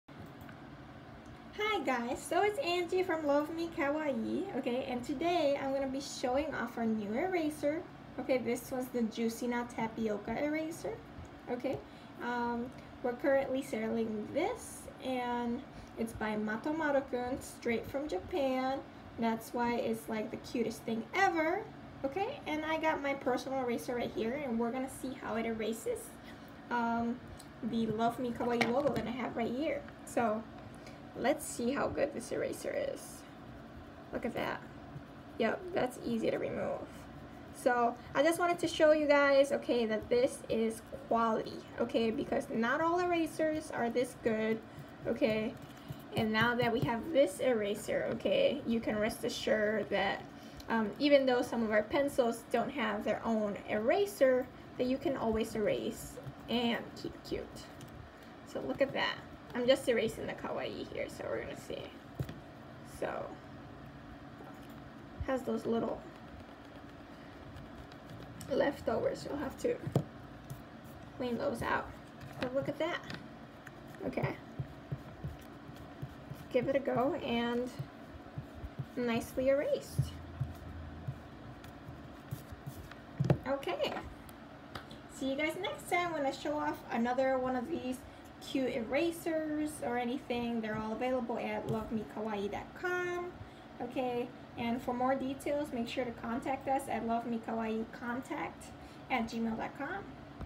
Kawaii matomarukun Juicy na Tapioca (bubble tea) Eraser Trial!!!
Watch The effectiveness of these Adorable  Bubble tea eraser by matomaru-kun. Available right now  At the lovemekawaii.com Shop!!